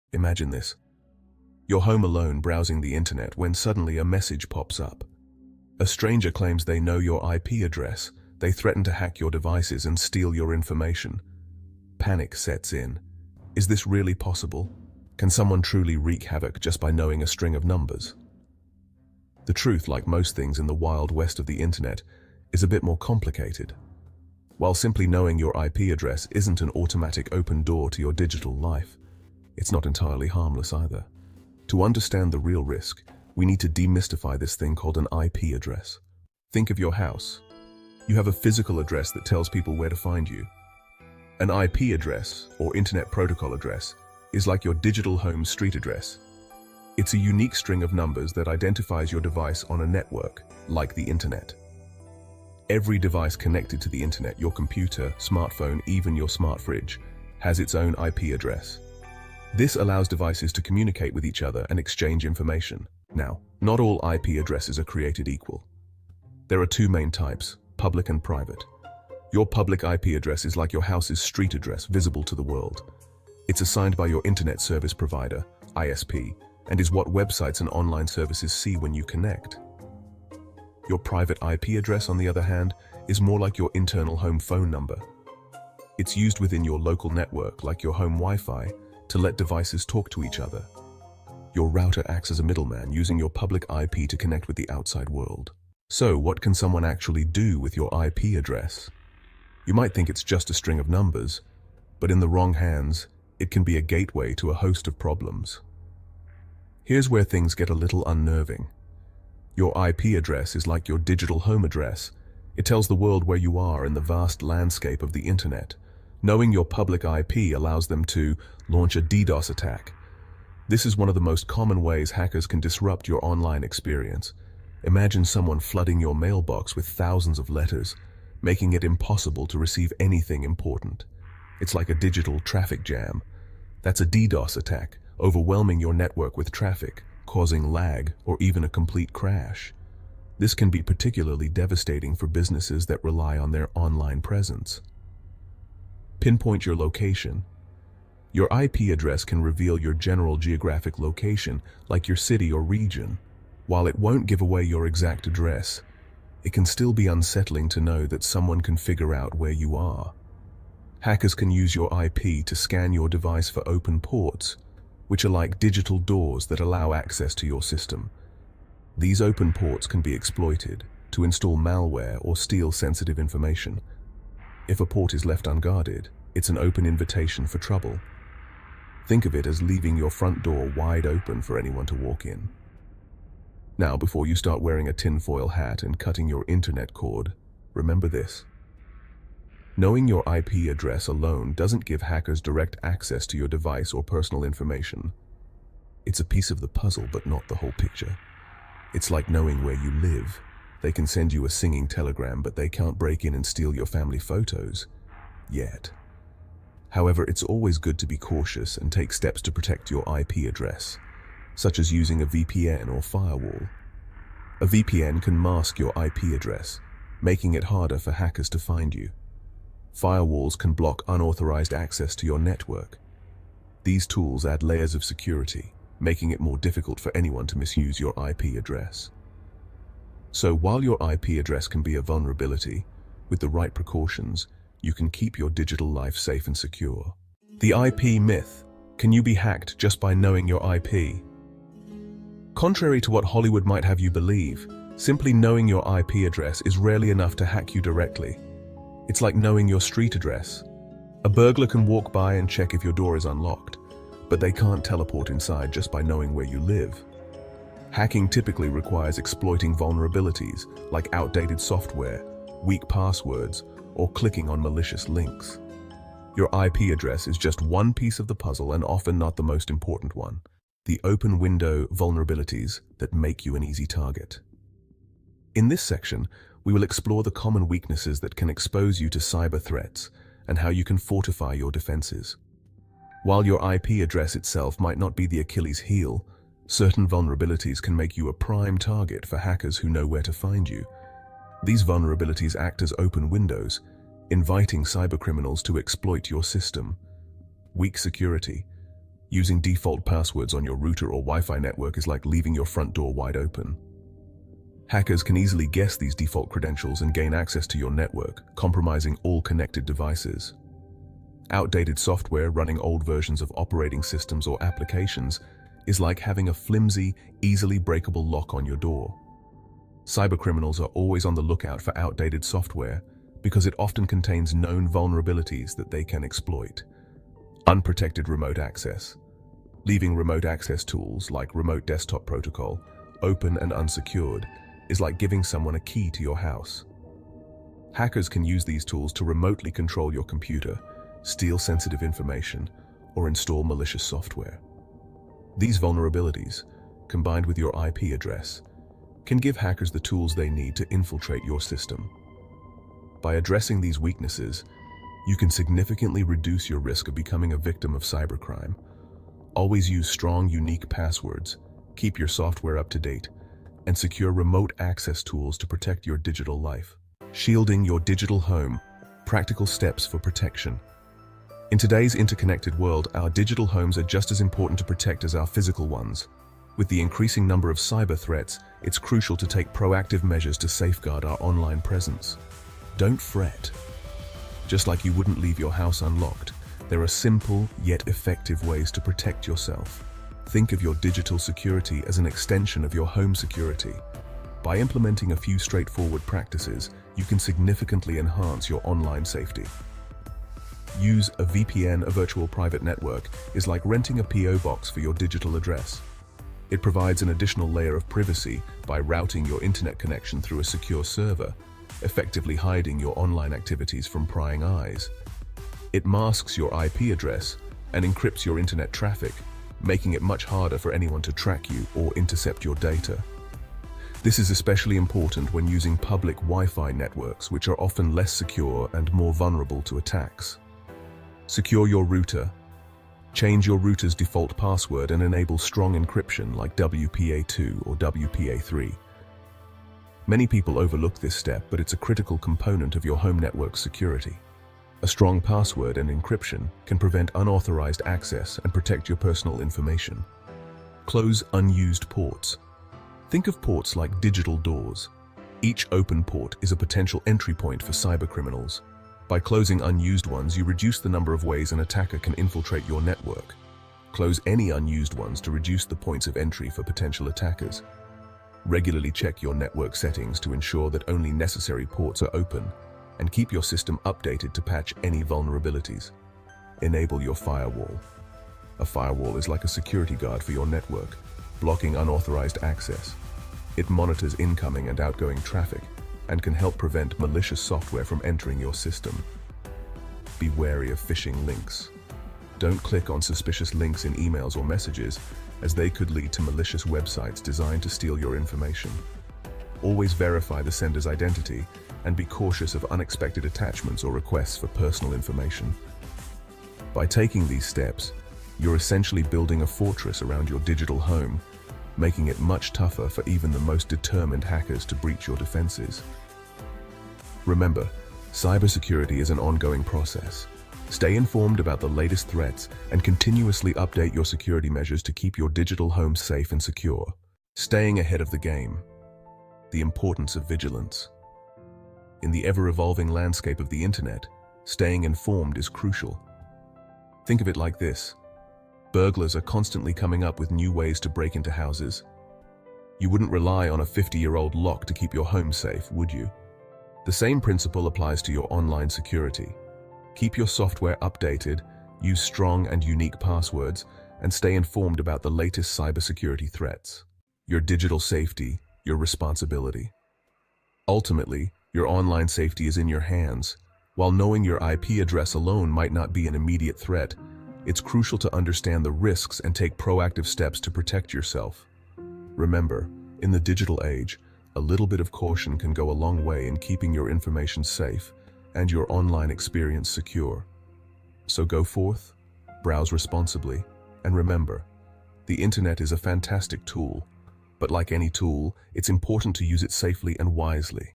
Can Someone Hack You by Just Knowing Your IP?
🚨 A Stranger Knows Your IP – Should You Worry? 🤔
A hacker claims to have your IP address and threatens to hack your devices – but how real is the danger? In this video, we break down everything you need to know about IP addresses, how they work, and what hackers can (and can’t) do with them. 🔥

🔍 What You'll Learn:
✅ What an IP Address is and how it functions 🌍
✅ Difference between Public & Private IPs 🔄
✅ How hackers misuse IP addresses ⚠️
✅ Common cyber threats like DDoS attacks & port vulnerabilities 💀
✅ Essential protection tips: VPNs, firewalls, and secure passwords 🔐

💡 Can hackers really hack you just by knowing your IP? Watch till the end to find out!

📌 OUTLINE:
⏱️ 00:00:00 A Knock on Your Digital Door 🚪
⏱️ 00:00:36 What is an IP Address? 🌐
⏱️ 00:01:03 Public vs. Private IPs 🔄
⏱️ 00:01:35 What Can Hackers Do with Your IP? 💀
⏱️ 00:03:57 Can You Be Hacked Just by Knowing Your IP? ❓
⏱️ 00:04:30 Vulnerabilities That Make You an Easy Target ⚠️
⏱️ 00:06:07 Practical Steps for Protection 🛡️
⏱️ 00:08:47 The Importance of Vigilance 👀
⏱️ 00:09:17 Your Digital Safety, Your Responsibility 🔑